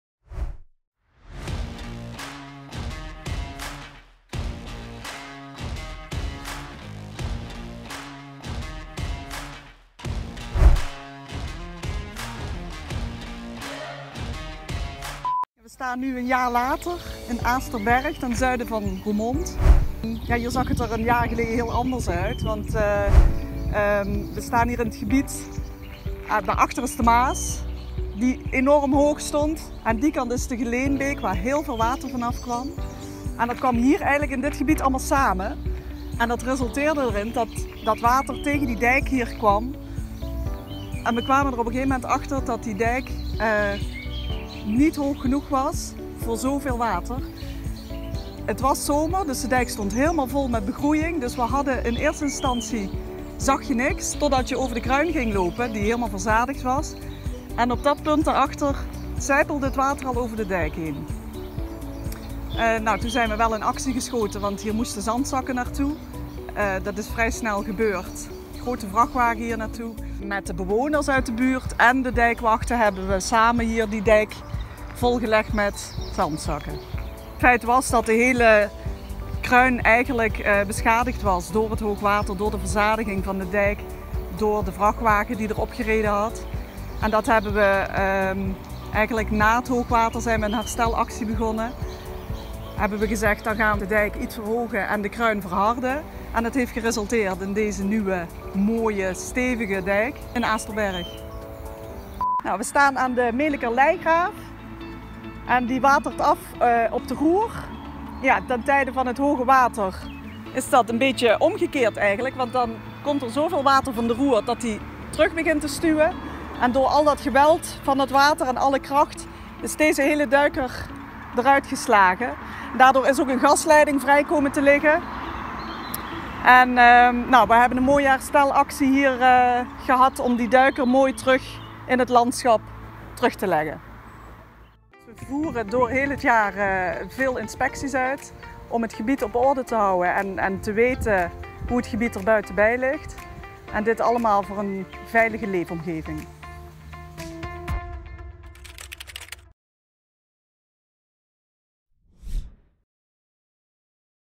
Roermond e.o.
We voeren het hele jaar inspecties uit om het gebied op orde te houden en te weten hoe het gebied erbij ligt. Dit doen we voor een veilige leefomgeving.

Samen met Rachel gaan we kijken in Roermond.